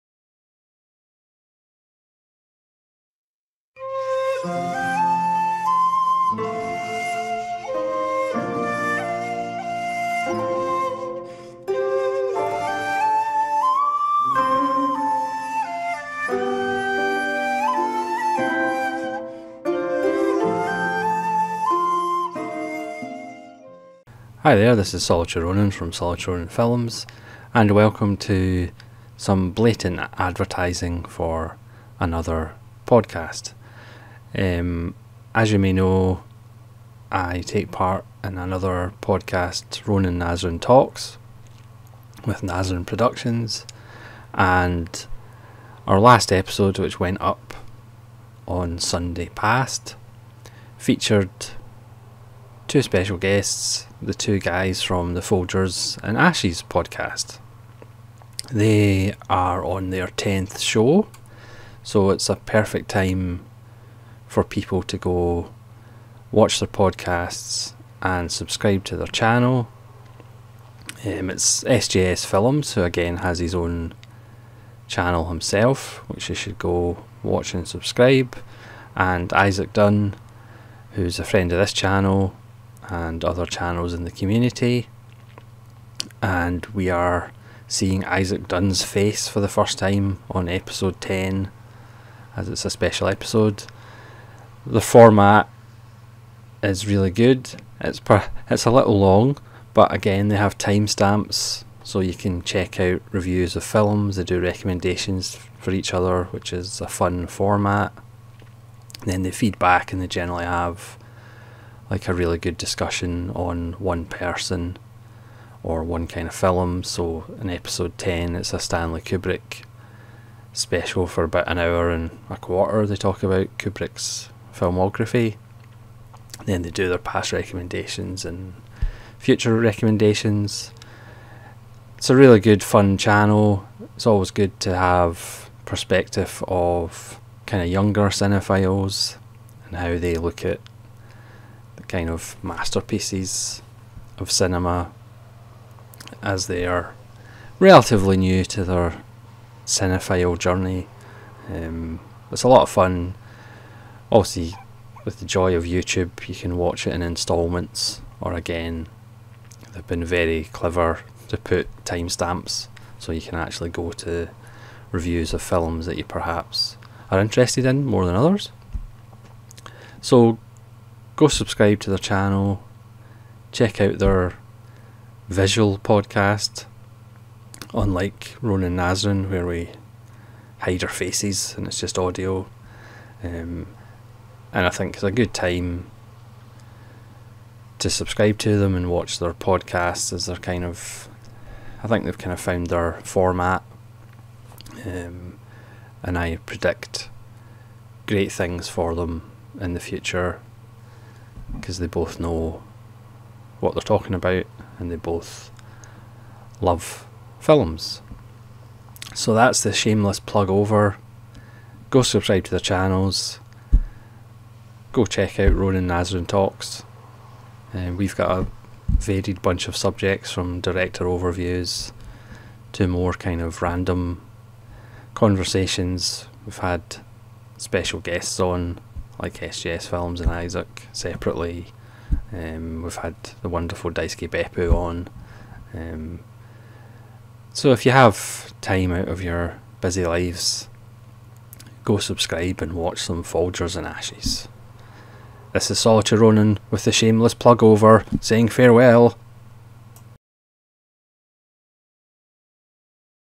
FOLGERS & ASHES PODCAST: GO TELL THE PEOPLE
Here's my ad for the folgers and ashes excellent podcast now hitting its stride at episode 10. You've got some catching up to do.